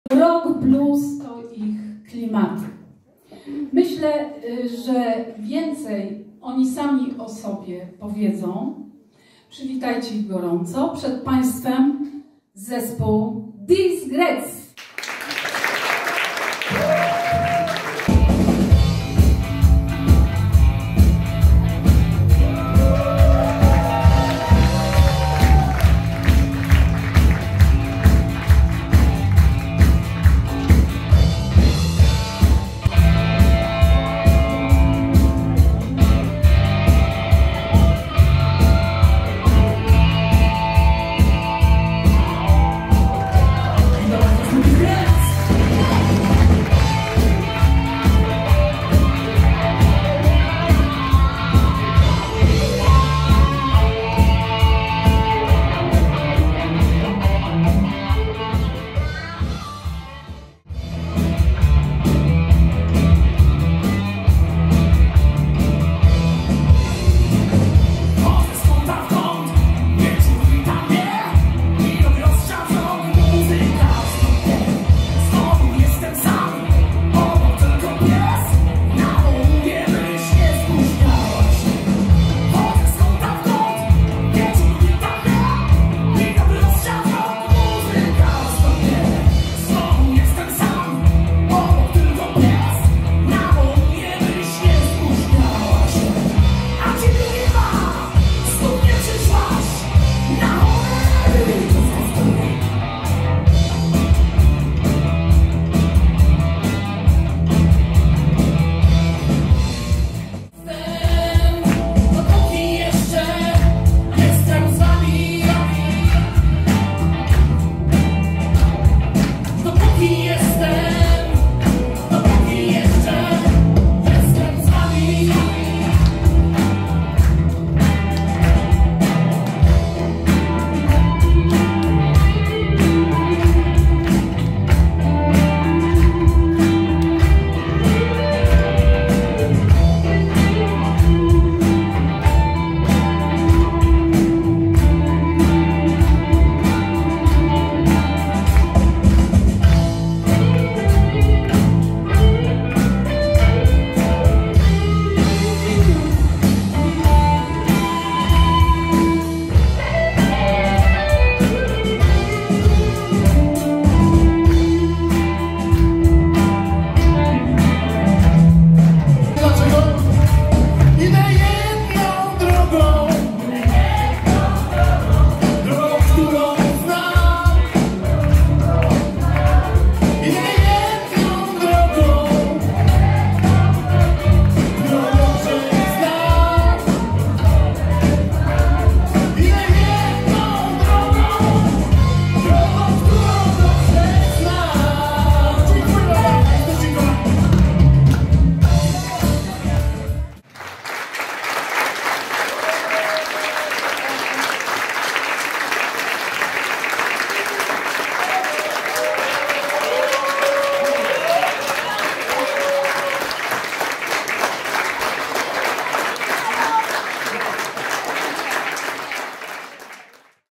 Dali czadu! The Zgreds zaszaleli na scenie Przystani!
The Zgreds to międzypokoleniowa grupa ludzi, których połączyła pasja i miłość do muzyki. Ich utwory są różne jak wrażliwość każdego członka kapeli – podkreślał frontman zespołu, Kamil. Na bardzo dobrze odbierany repertuar Zgredów, składają się wyłącznie autorskie kawałki – a ciekawe aranżacje połączone z dobrym tekstem to gwarancja sukcesu. Tekstami w zespole zajmują się Eliza Cisse, Jacek Sulkowski i Sławomir Wojciechowski. Kompozycje to dzieło Ani Kubiak, Elizy Cisse, Jacka Sulkowskiego i Sławomira Wojciechowskiego. Wokal – Eliza i Kamil Mączka, na bębnach, po 30 latach przerwy, gra Dariusz Korycki, na basie – Ania Kubiak. Nad całością czuwa akustyk Hubert Janicki.